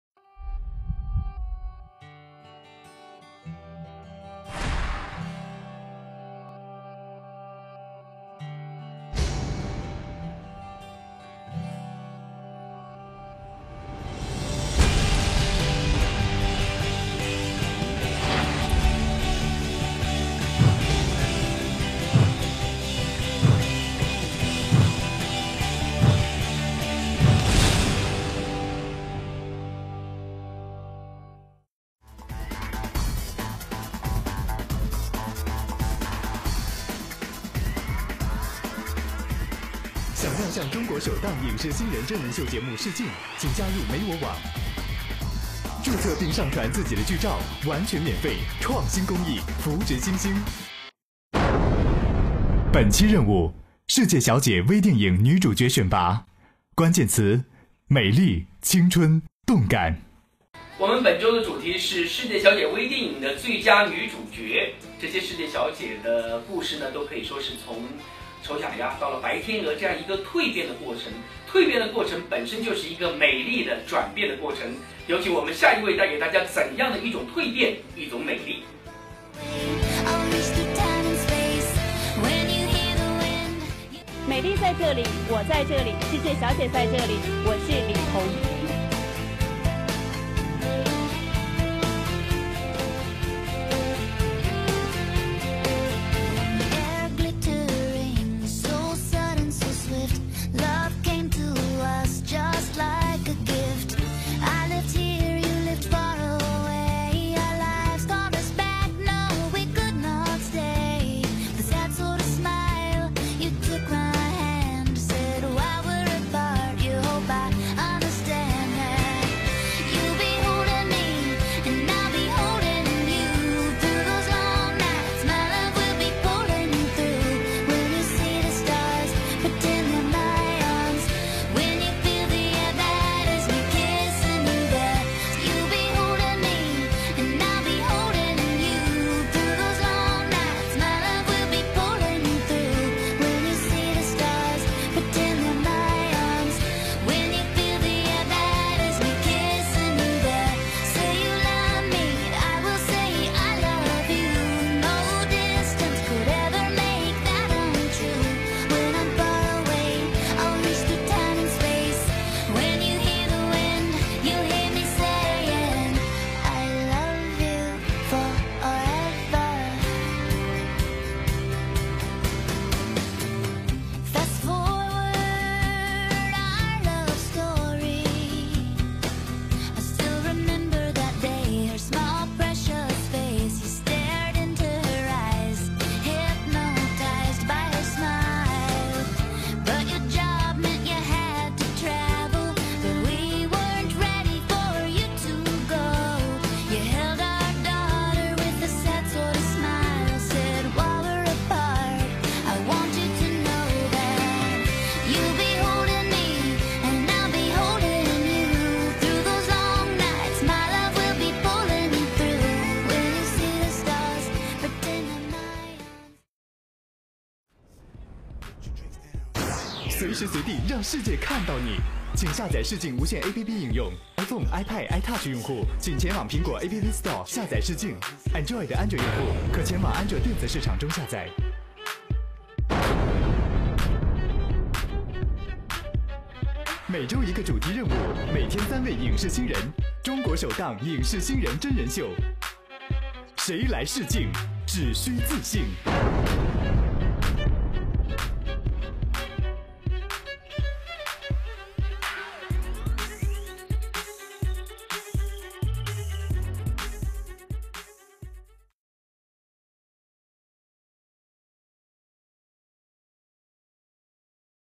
[Ku6 Micro Film]Audition---micro movie actress selection about Miss World 2-3 LiTong
You can see the process of the micro movie actress selection  . wait and see what happens!